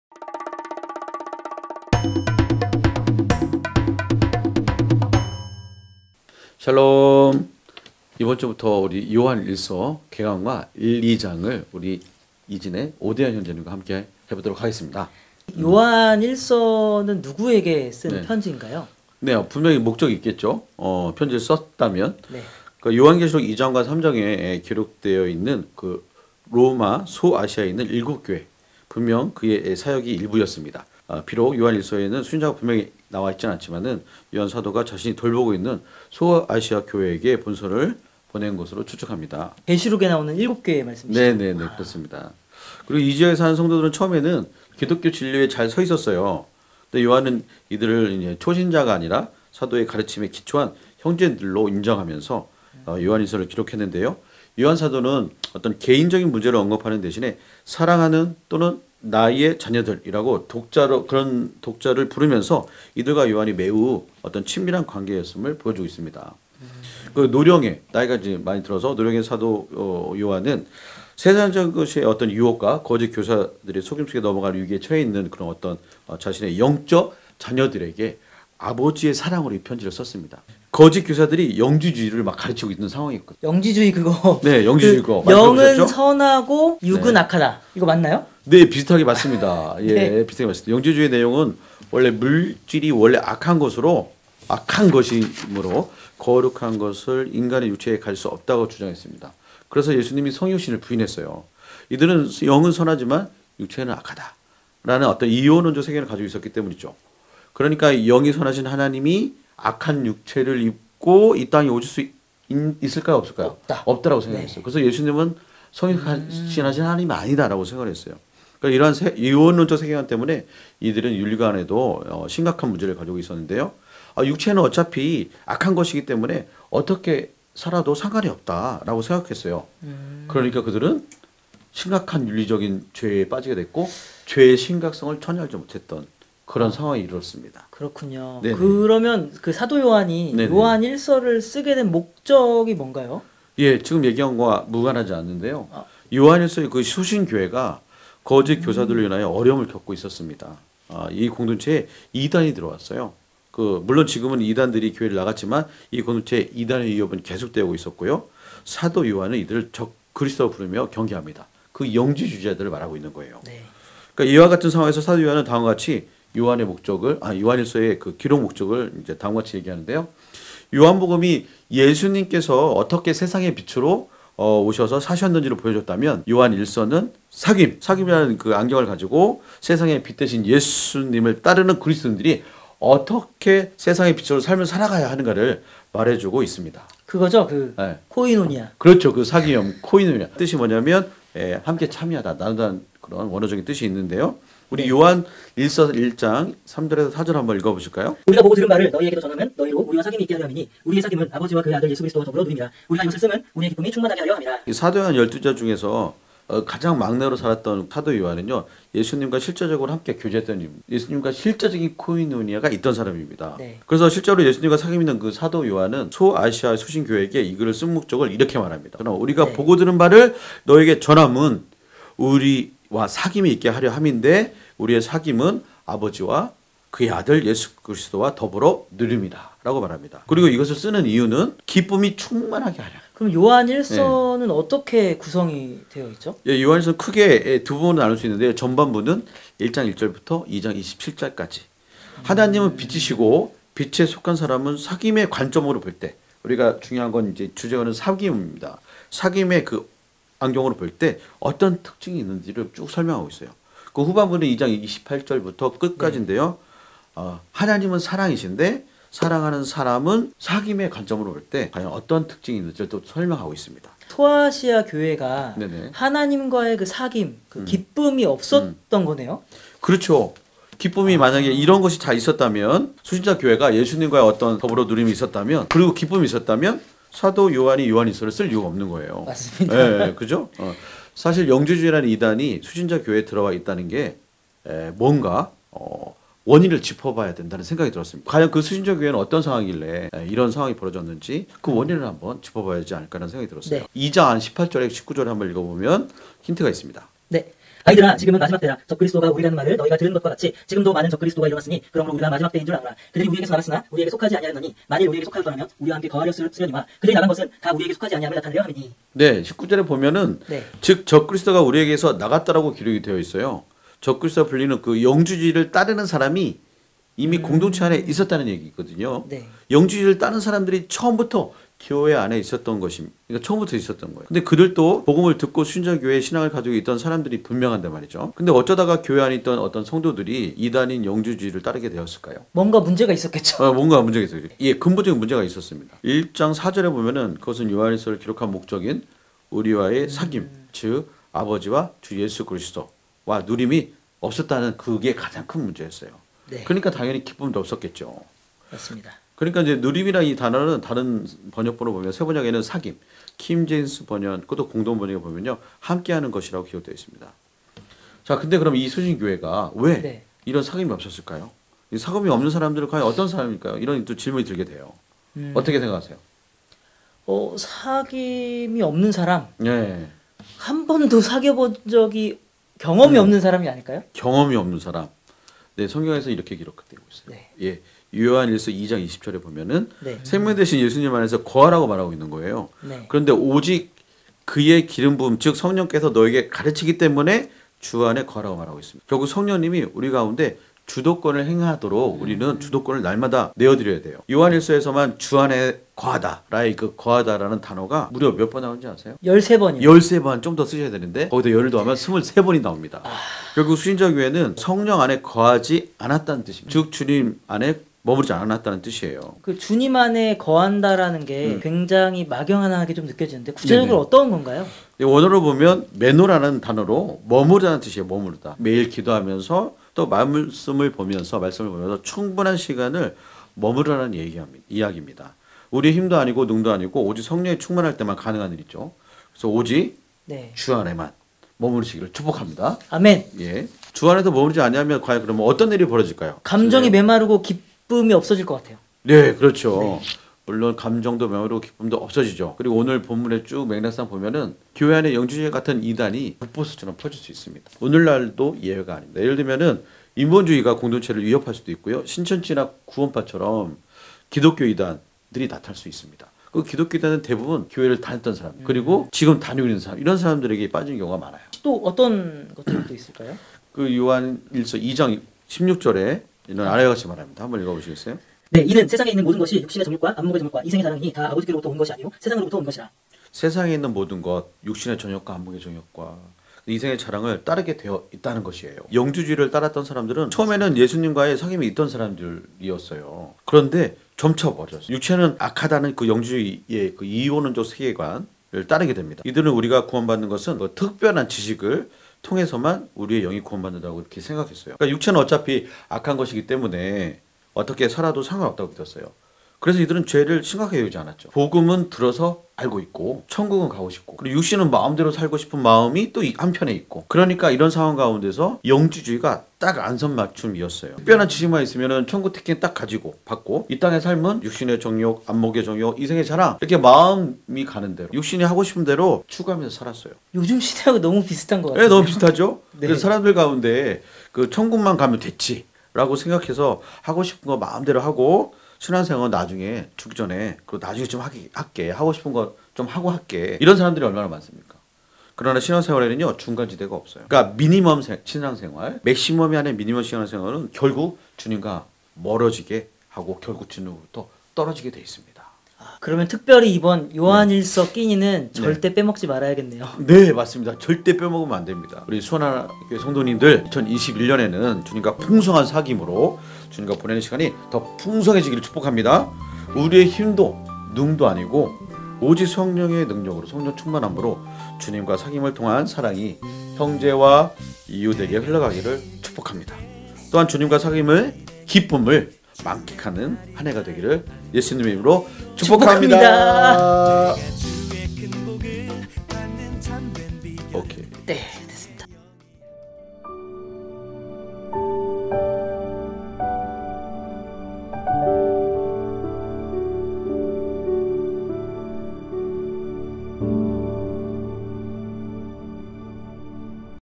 요한일서 개관, 1-2장 | 하나님과 참된 사귐을 통하여, 현대판 영지주의를 몰아내자!!
요한일서 말씀묵상을 통하여 하나님과의 참된 사귐이 회복되고, 현대판 영지주의가 여러분 가운데 발 붙이지 못하도록 몰아내시기를 축복합니다.

*끼니[명사] :
아침, 점심, 저녁과 같이
날마다 일정한 시간에 먹는 밥.

본 영상은 수원하나교회 말씀 묵상집 '끼니'를 묵상할 때
말씀을 보는 관점과 이해에 도움을 드리고자 제작되었습니다.